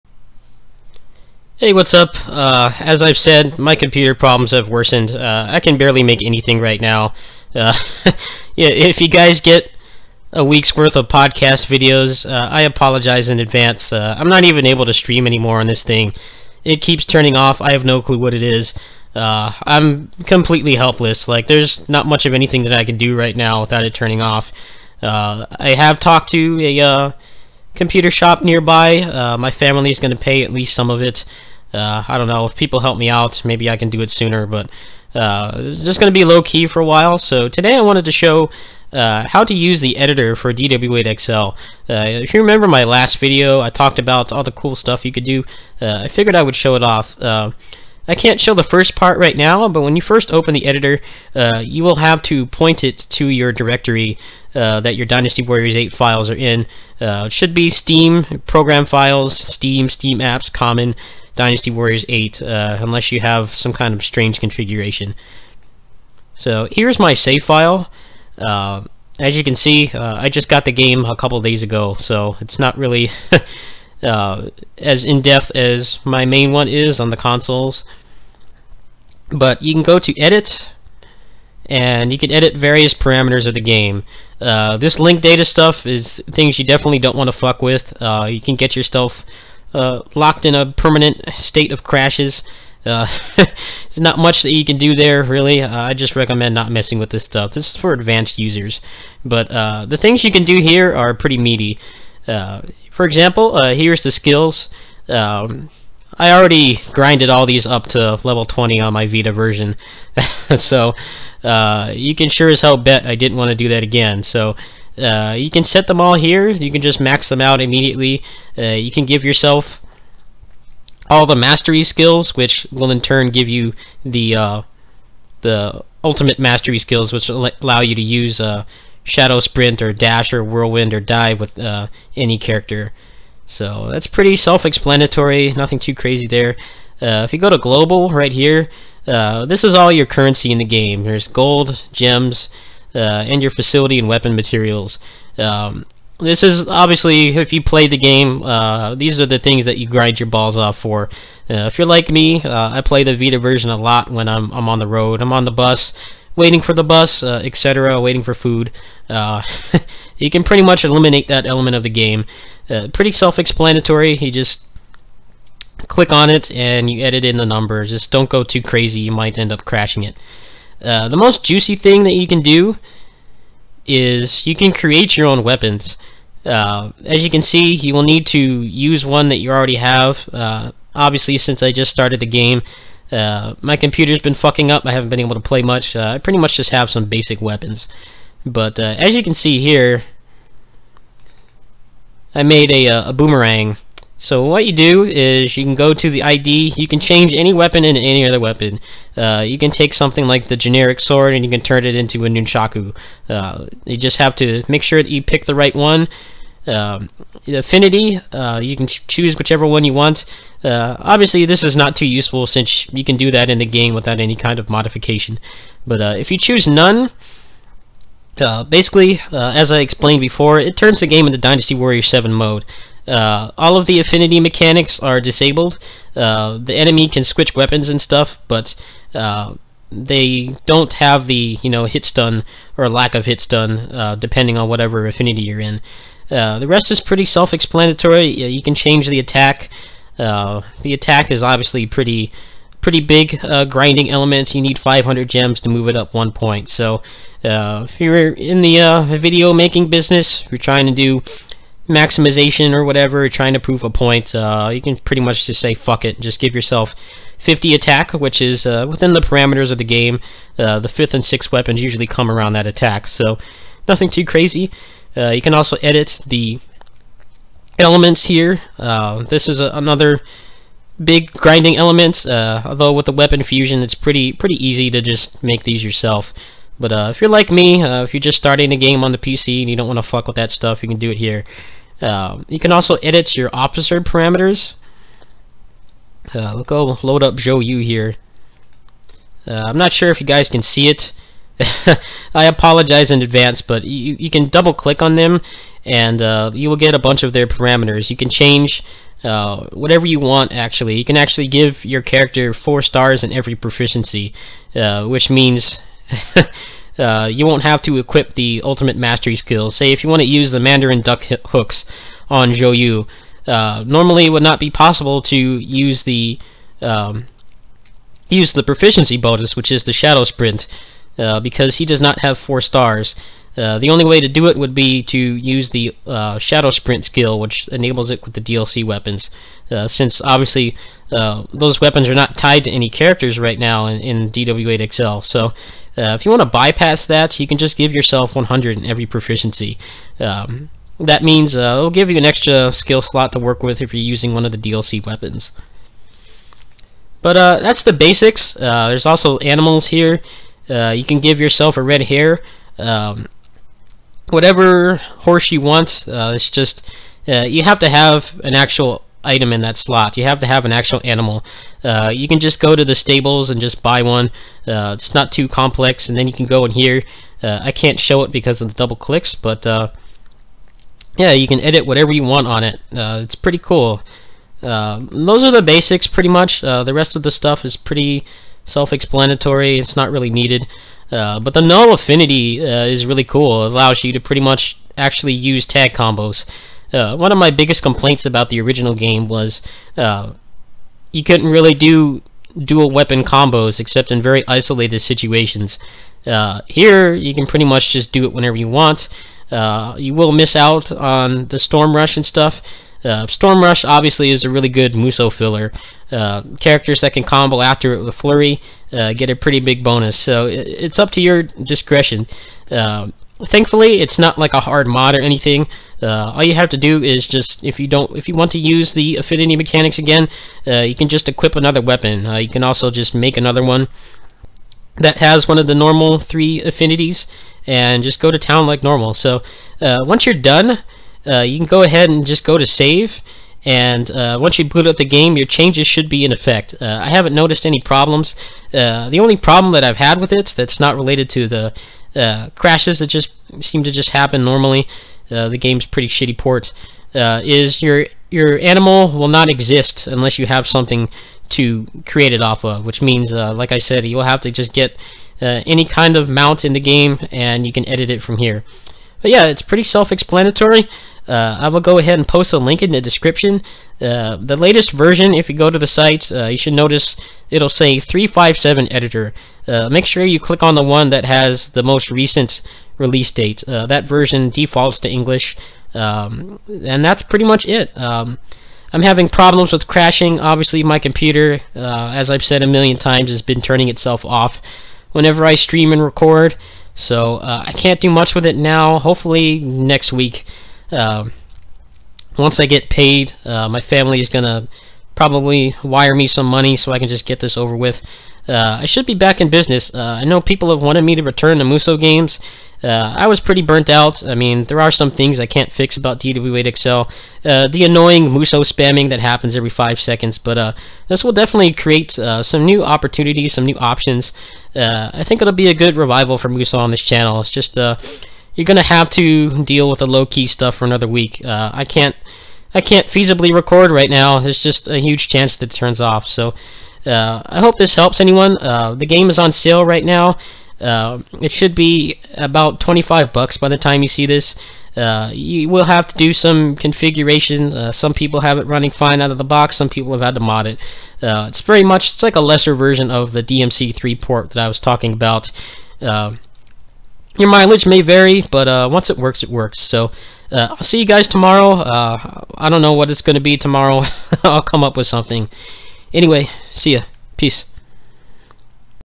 How to use Van's DW8XL PC Save Editor
It is titled "357Editor". Make sure to pick the most recent version.

If you want to help fund my PC repairs and/or other projects, My Paypal is